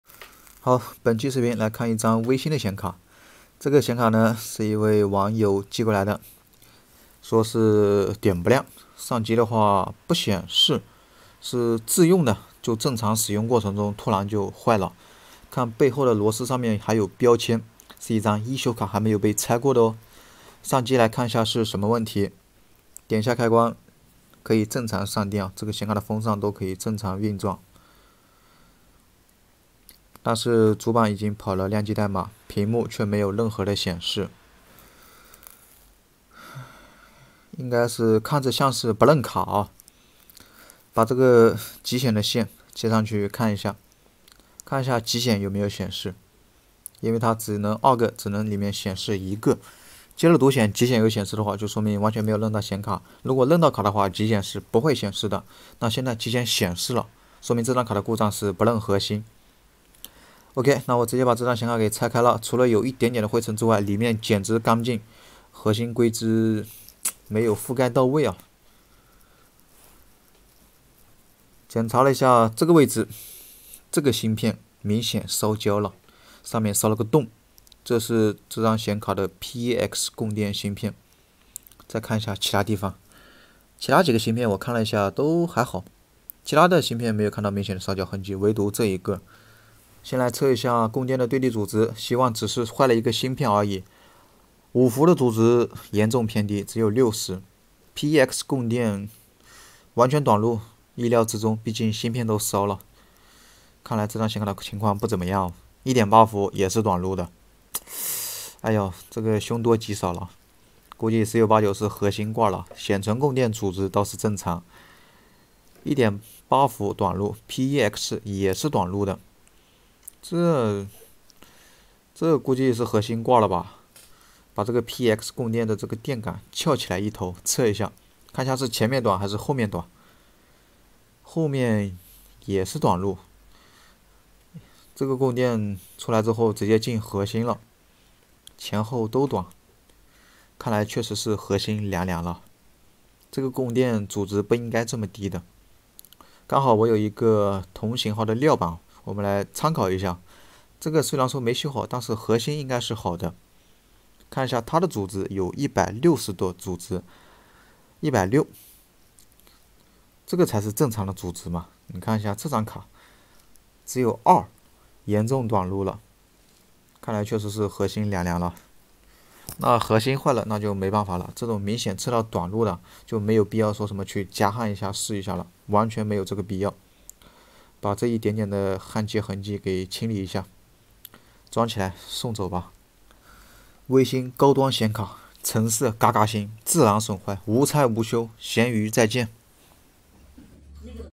【显卡维修】微星高端显卡全线短路，核心损坏，价值暴跌#靓仔修机
我们的视频是为了为观众提供娱乐，同时也保证了拍摄过程的安全。 【显卡维修】微星高端显卡全线短路，核心损坏，价值暴跌#靓仔修机